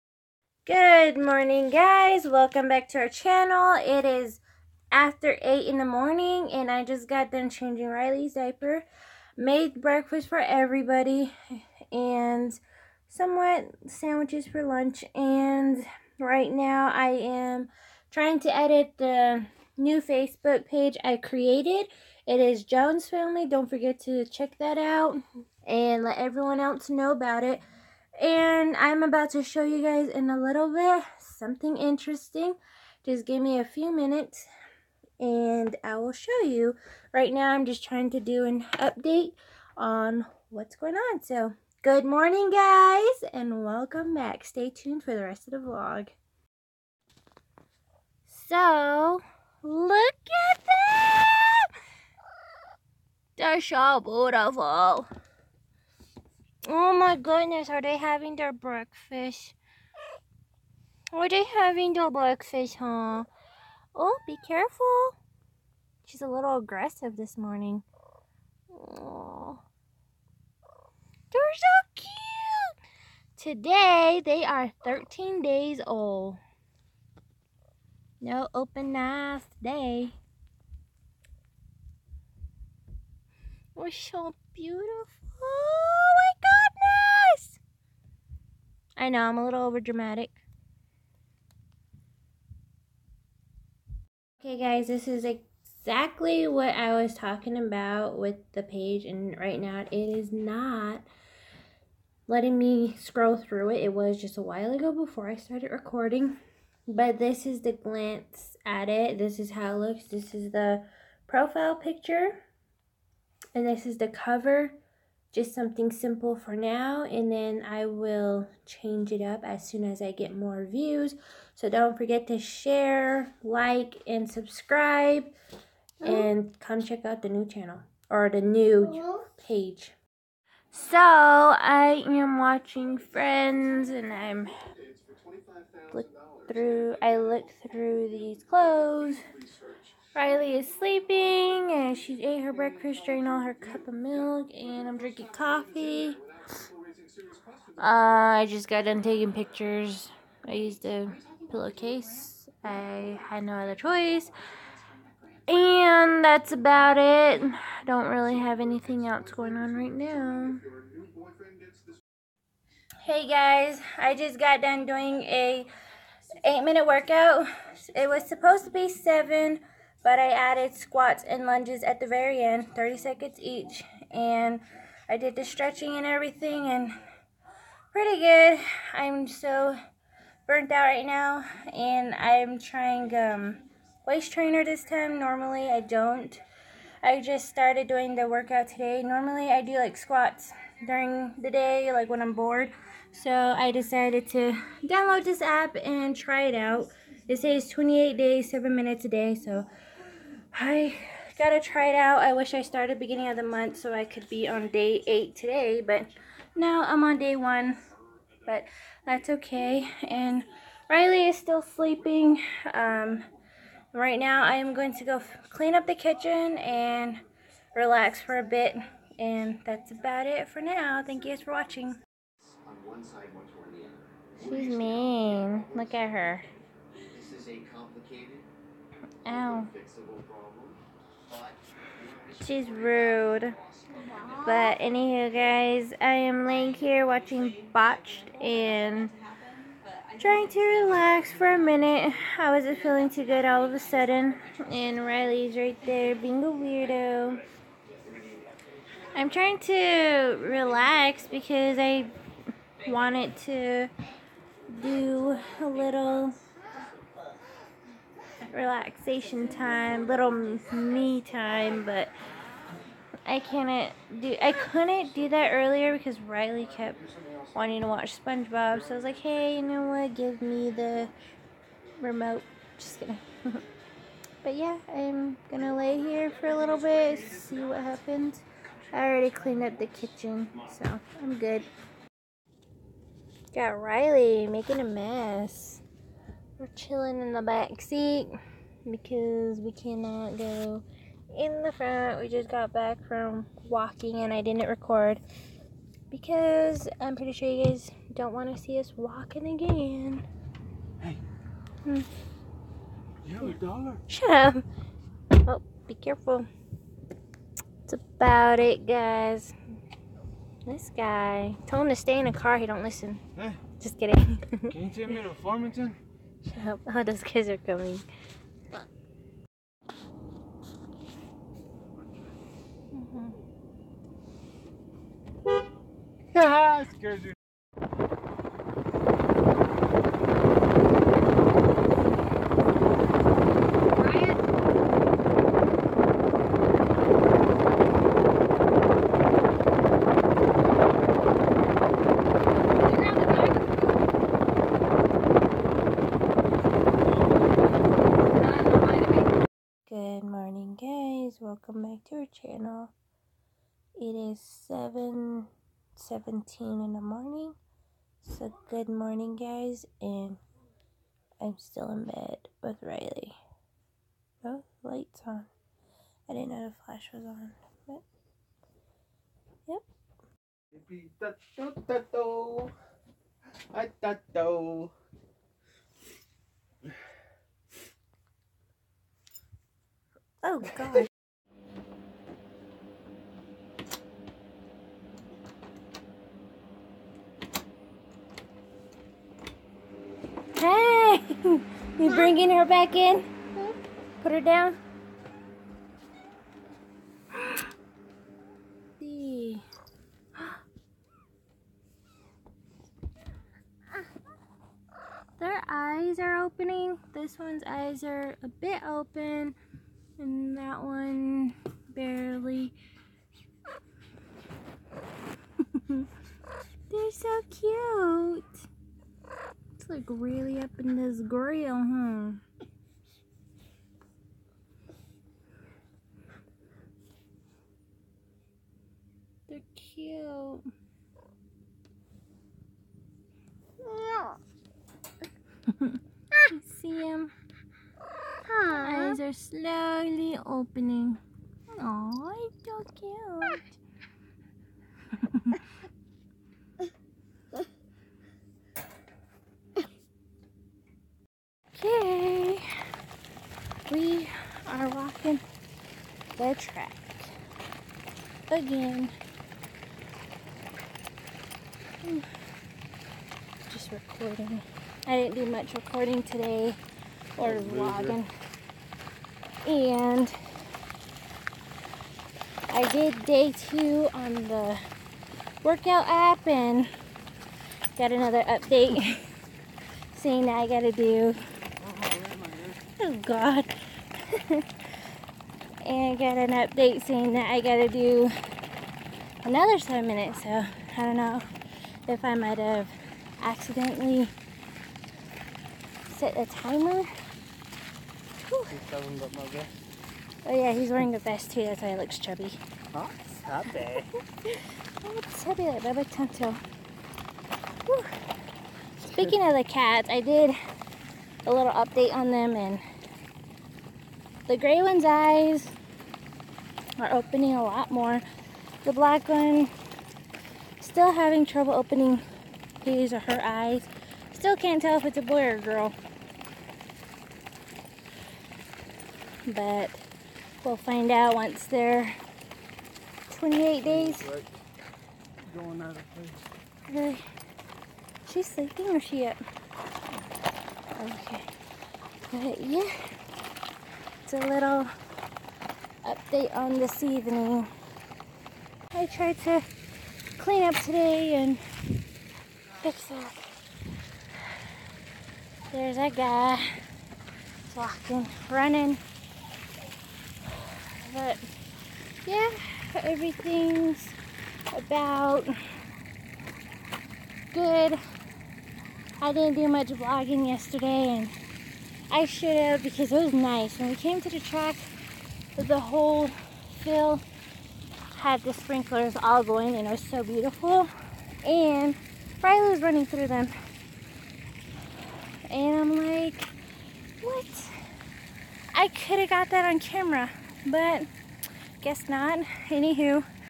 BEING LAZY.....
Update on the kittens & I'm sorry I didn't vlog alot. Today's Vlog is 3days in one due to me not recording a lot and that's my fault.